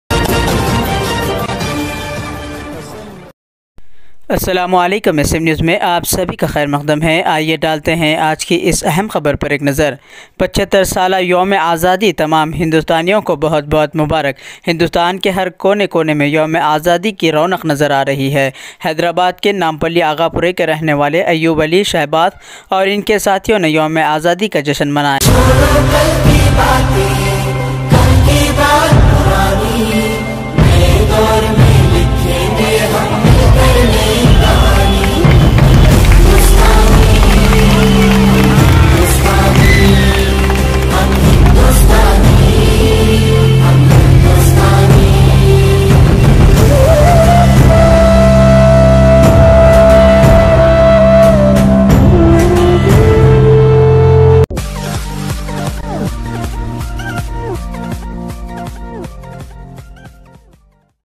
75 Independent Day celebration Nampally Aghapura Rahane wale Ayub Shahbaz Ali aur unke Sathiyon ne
75 Independent Day celebration Nampally Aghapura Rahane wale Ayub Shahbaz Ali aur unke Sathiyon ne Milkar yome Azadi ke jasn Manaya